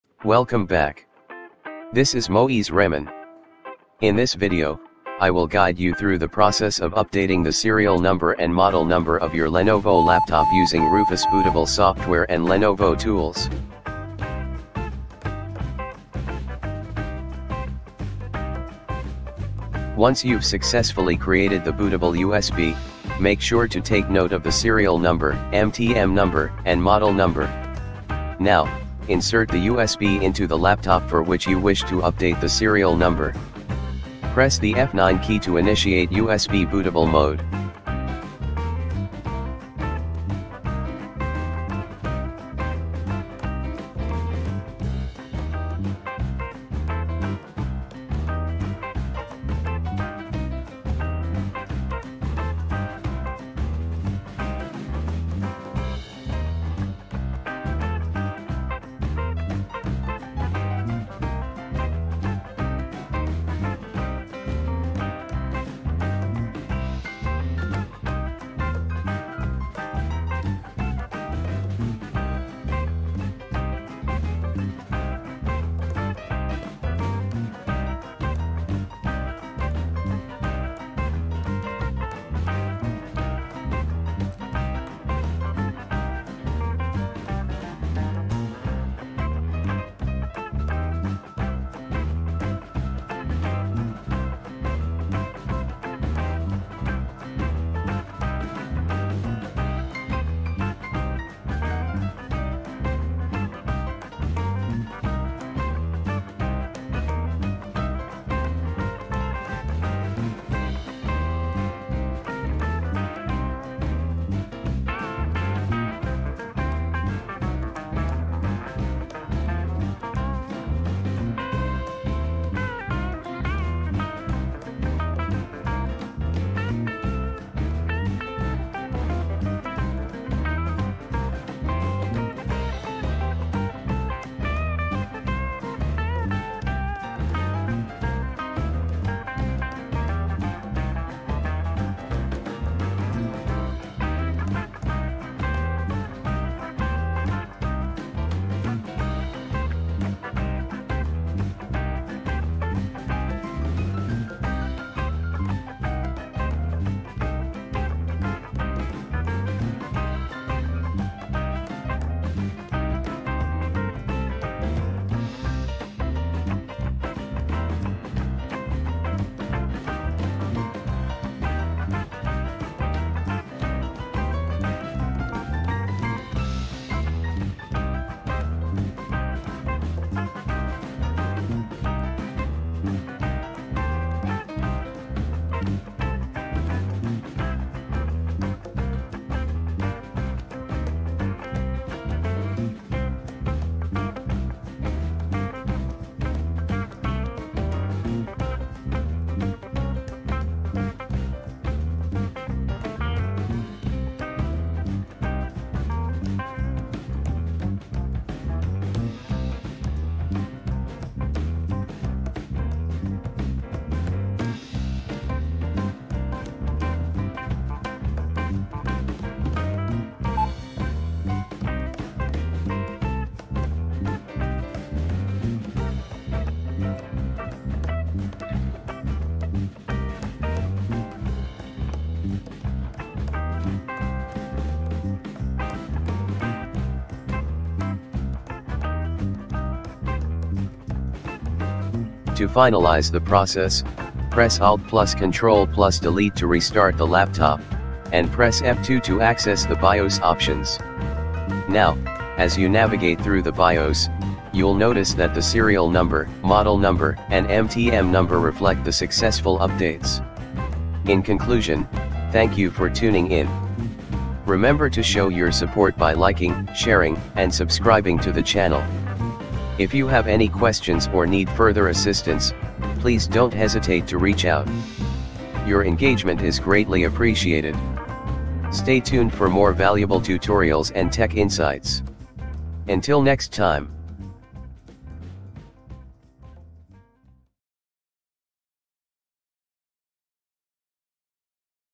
How to Update Serial Number Lenovo Invalid Error || ThinkPad E15 Gen 5 | Model, UUID Error
In this video, I will guide you through the process of updating the serial number and model number of your Lenovo laptop using Rufus bootable software and Lenovo tools.

Sorry F12 key for lenovo boot,

ThinkPad E15 Gen 4 12th Generation
13th Generation

lenovo serial number invalid error || ThinkPad, ThinkCentre, IdeaPad, Yoga, lenovo legion Series,